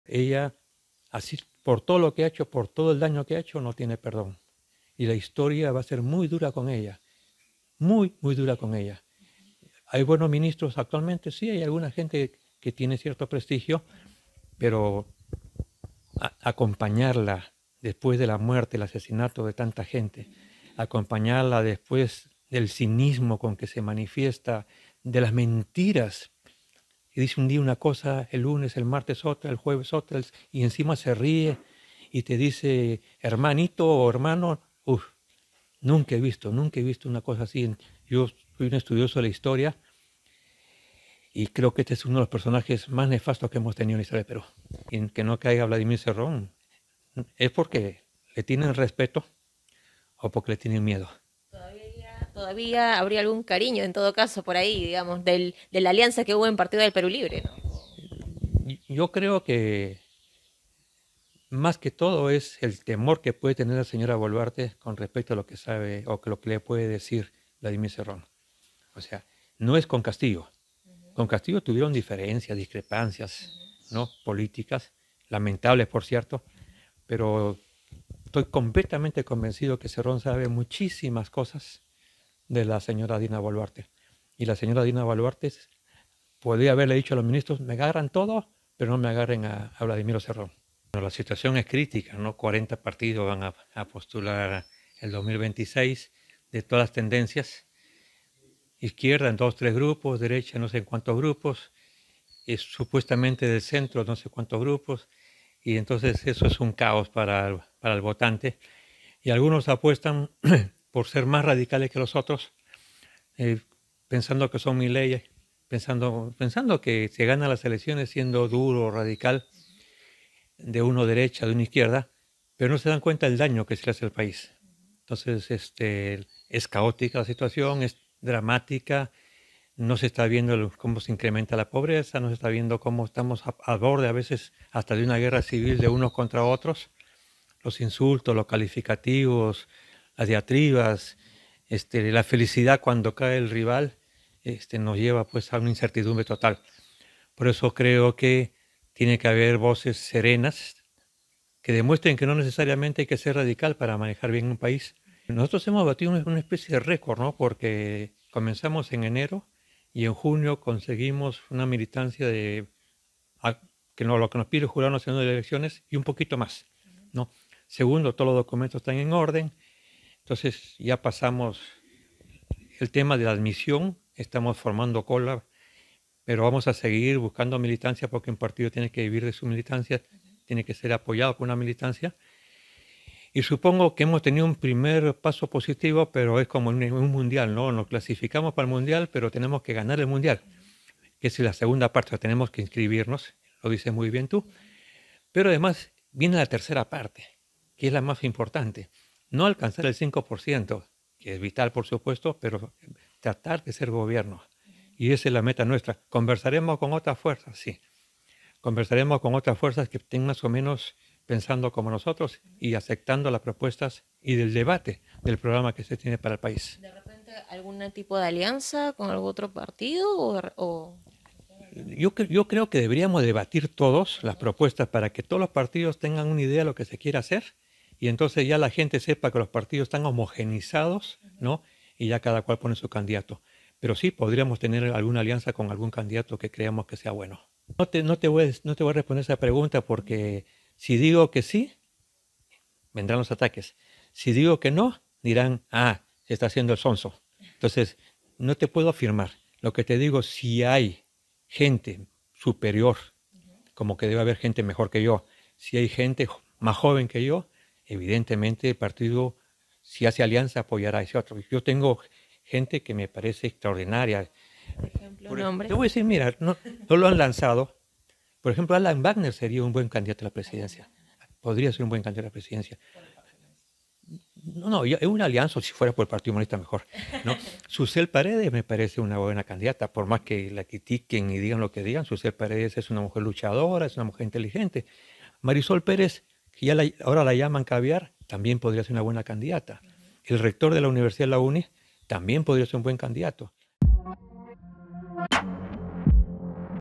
Yehude Simon: "BOLUARTE es de los PERSONAJES MÁS NEFASTOS que hemos tenido"
El presidente del Partido Humanista Peruano, Yehude Simon, sostiene que habría temor en el Gobierno para capturar al prófugo líder de Perú Libre, Vladimir Cerrón.